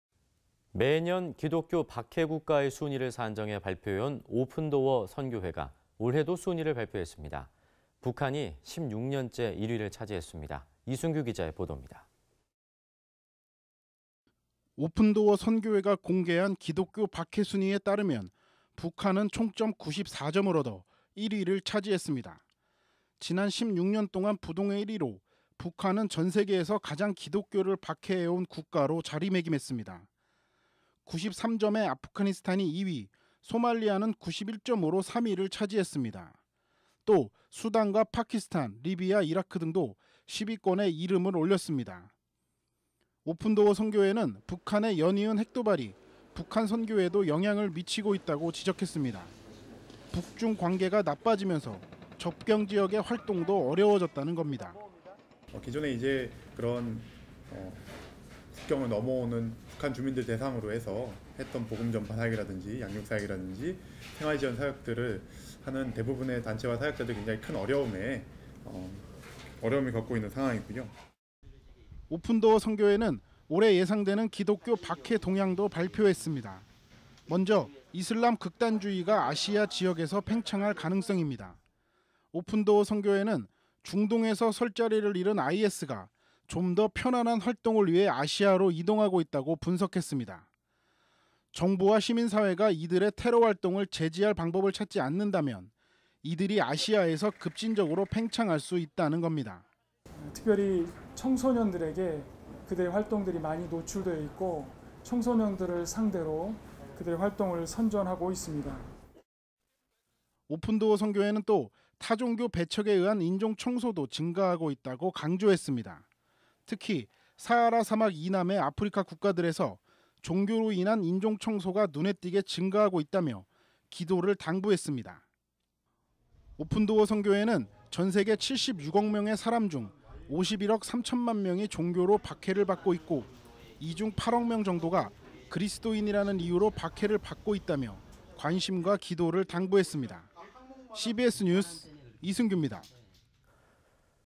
[CBS뉴스] 오픈도어 선교회, 기독교 박해 순위 공개
[앵커]

매년 기독교 박해 국가의 순위를 산정해
발표해온 오픈도어 선교회가
올해도 순위를 발표했습니다.

북한이 16년째 1위를 차지했습니다.

이승규 기자의 보돕니다.

[기자]

오픈도어 선교회가 공개한 기독교 박해 순위에 따르면
북한은 총점 94점을 얻어 1위를 차지했습니다.

지난 16년 동안 부동의 1위로,
북한은 전 세계에서 가장 기독교를 박해해온
국가로 자리매김했습니다.

93점의 아프가니스탄이 2위,
소말리아는 91점으로 3위를 차지했습니다.
또, 수단과 파키스탄, 리비아, 이라크 등도
10위권에 이름을 올렸습니다.

오픈도어 선교회는
북한의 연이은 핵 도발이
북한 선교에도 영향을 미치고 있다고 지적했습니다.

북중관계가 나빠지면서
접경 지역의 활동도 어려워졌다는 겁니다.

이 다니엘 간사 / 오픈도어 선교회
"기존에 국경을 넘어오는 북한 주민을 대상으로
했던 복음 전파 사역이나 양육 사역이나
생활지원 사역들을 하는 대부분의 단체와 사역자들이
큰 어려움을 겪고 있는 상황이고요.."

(장소) 오픈도어 선교회 기독교 박해 순위 공개
/ 오늘(어제), 서울 타타이키친

또 올해 예상되는 기독교 박해 동향도 발표했습니다.

먼저 이슬람 극단주의가
아시아 지역에서 팽창할 가능성입니다.

중동에서 설 자리를 잃은 IS가
좀 더 편안한 활동을 위해
아시아로 이동하고 있다고 분석했습니다.

정부와 시민사회가
이들의 테러 활동을 제지할 방법을 찾지 않는다면,
이들이 아시아에서 급진적으로
팽창할 수 있다는 겁니다.

[녹취] [녹취] 이종만 목사 / 오픈도어 선교회 사무총장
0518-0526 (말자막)
"특별히 청소년들에게 그들의 활동이
많이 노출되어 있고 청소년들을 상대로
그들의 활동을 선전하고 있습니다."

또 타종교 배척에 의한
인종 청소도 증가하고 있다고 강조했습니다.

특히 사하라 사막 이남의
아프리카 국가들에서 종교로 인한
인종청소가 눈에 띄게 증가하고 있다며
기도를 당부했습니다.

지구상 76억 명의 사람 중
51억 3천만 명이 종교로 박해를 받고 있고,
이중 8억 명 정도가
그리스도인이라는 이유로 박해를 받고 있다며,
관심과 기도를 당부했습니다.

CBS 뉴스 이승규입니다.
영상 취재 최현 영상 편집